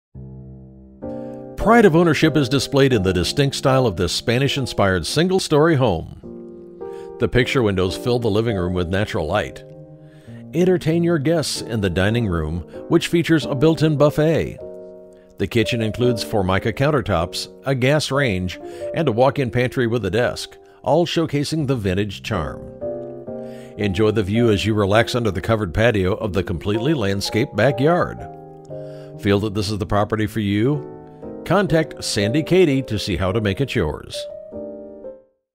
1959 Main Street Riverside, CA
1959 Main Street, Riverside, CA

You will love the distinct style of this Spanish-Inspired single-story home. Pride of ownership is displayed in this sweet, move-in ready home. Streams of natural light flowing through the front picture windows fill the large living room. Entertain your guest in the formal dining room which features